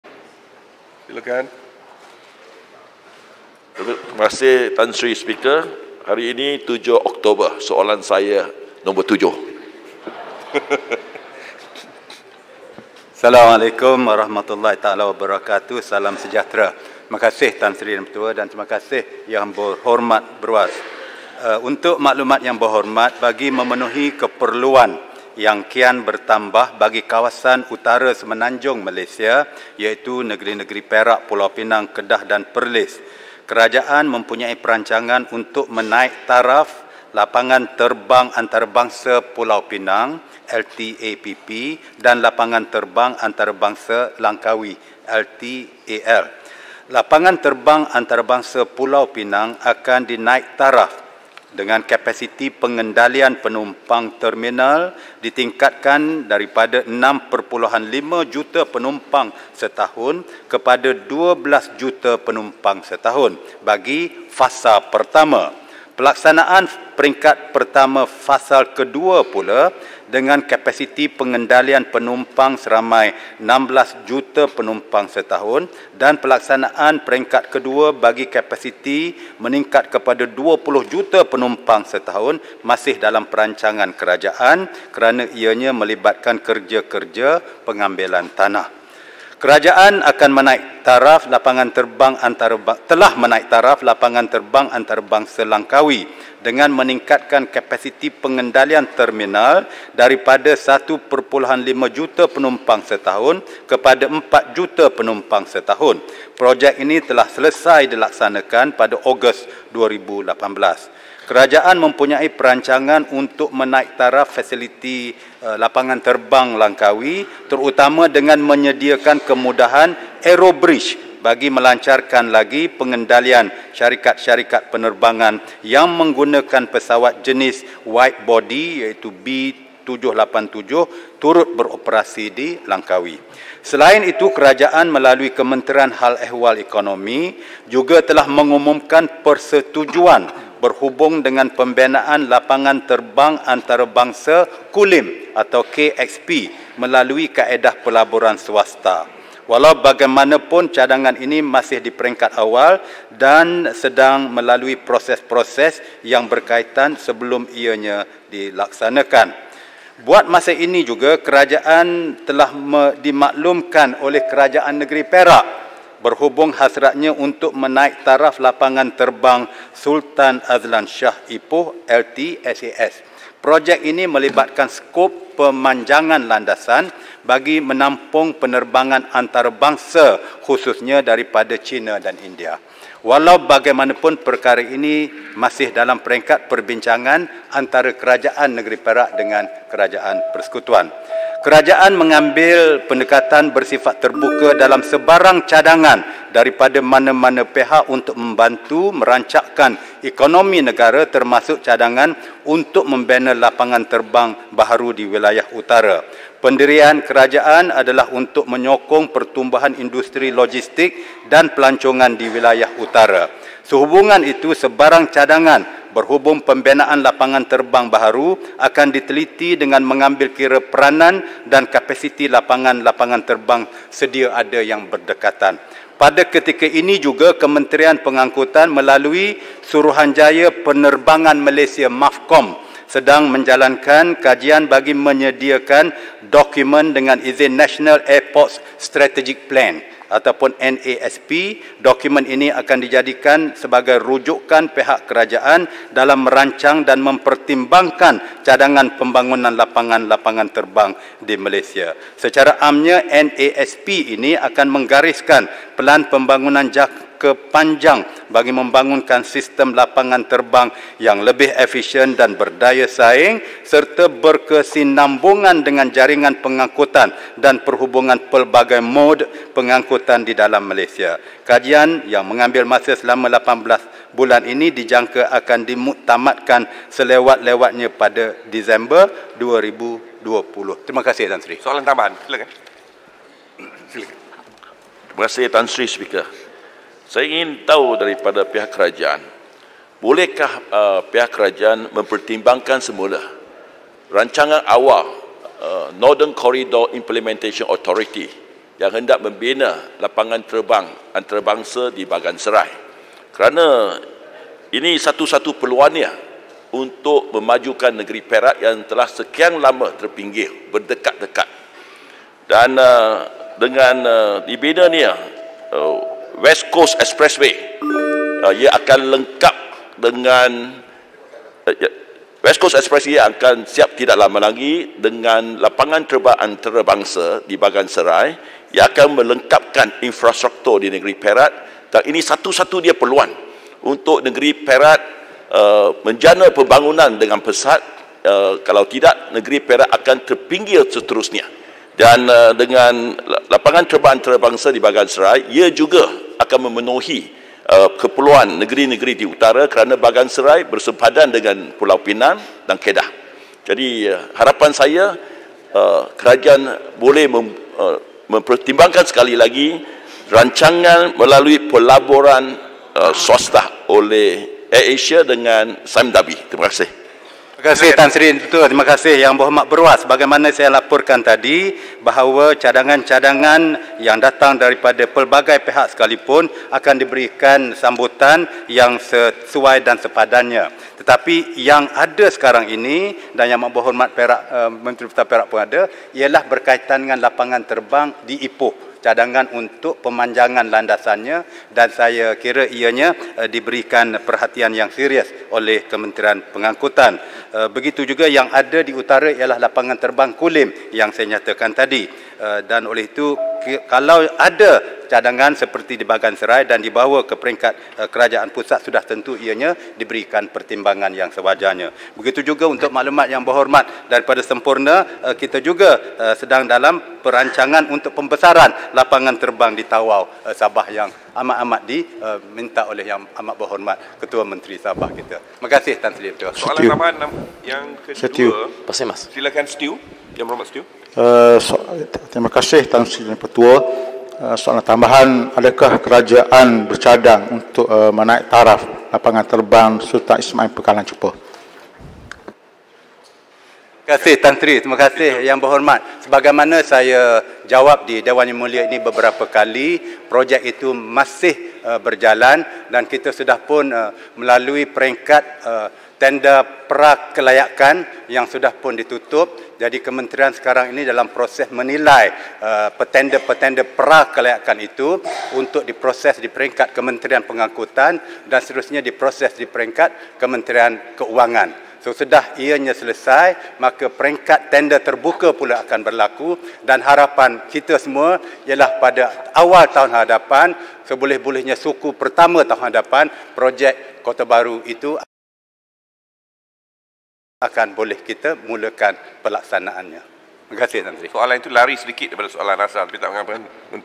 Ngeh Koo Ham - Apa rancangan Kerajaan untuk memperluas lapangan terbang?
Dato' Ngeh Koo Ham [ Beruas ] minta MENTERI PENGANGKUTAN menyatakan rancangan Kerajaan untuk memperluas lapangan terbang atau membina lapangan terbang baru untuk memenuhi keperluan yang kian bertambah bagi kawasan utara Semenanjung Malaysia, iaitu negeri Perak, Pulau Pinang, Kedah dan Perlis.